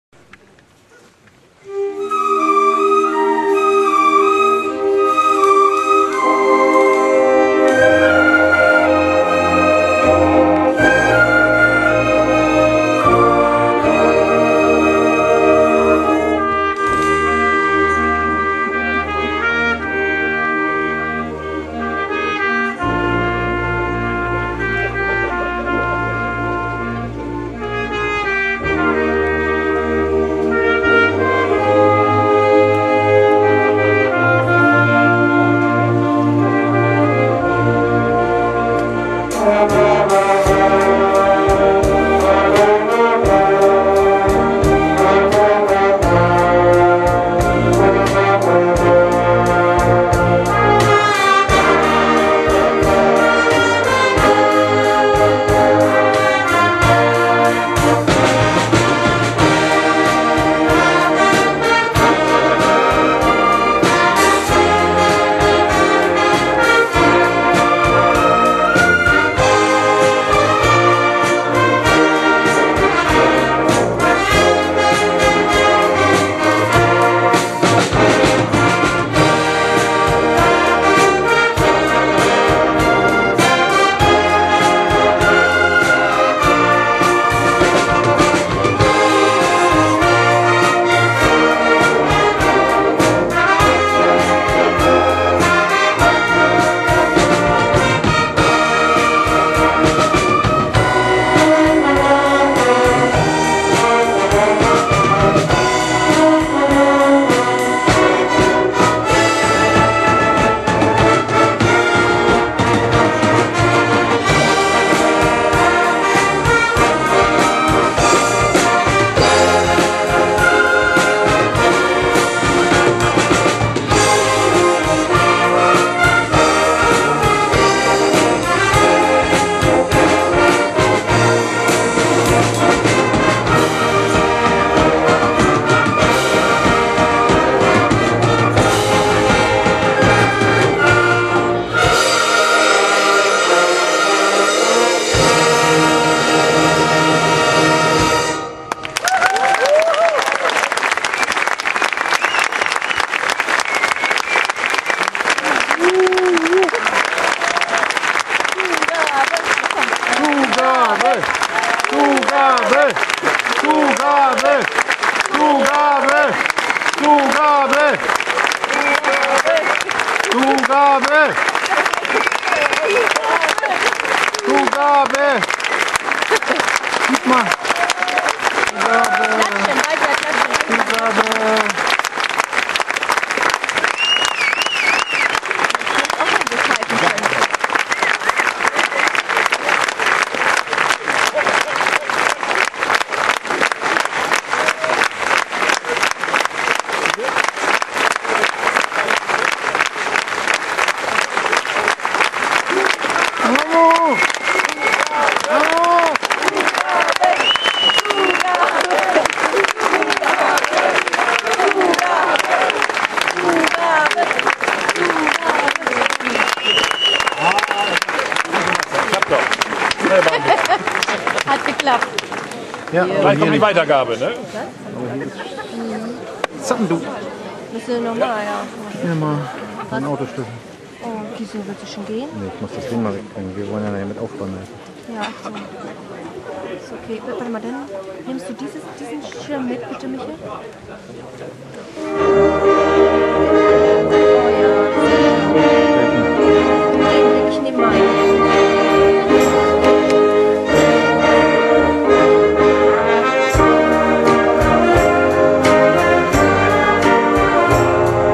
BLASMUSIK OPEN AIR Seebühne Bremervörde FINALE
Moderne und traditionelle Blasmusik auf der Seebühne in Bremervörde
Am Sonntag den 15. Juli 2012 wurde die Seebühne zur großen Orchesterbühne. Erstmals konzertierten die großen Orchestergruppen der Oste-Stadt gemeinsam.   Krönender Abschluss war, als alle Gruppen gemeinsam auf der Bühne zum Finale spielten.